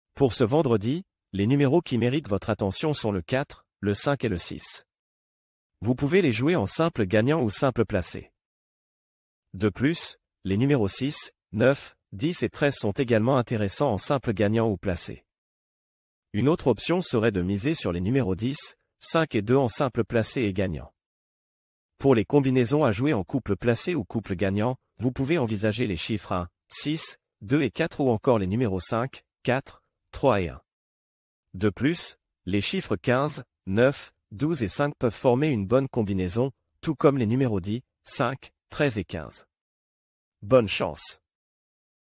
Vendredi en courses
" Vendredi " ,
numeros ou valeurs interessantes du vendredi ,
c est 4-5-6
ou
les numeros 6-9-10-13 en simples gagnant simples places
sinon
 les numeros 10-5-2 en simples places simples  "gagnant " .

les combinaisons sinon a jouer couple place ou couple ggt voir en formule 2/4
sont 1-6-2-4
ou 5-4-3-1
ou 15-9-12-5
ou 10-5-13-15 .

3 series de numeros sinon 4 combinaisons .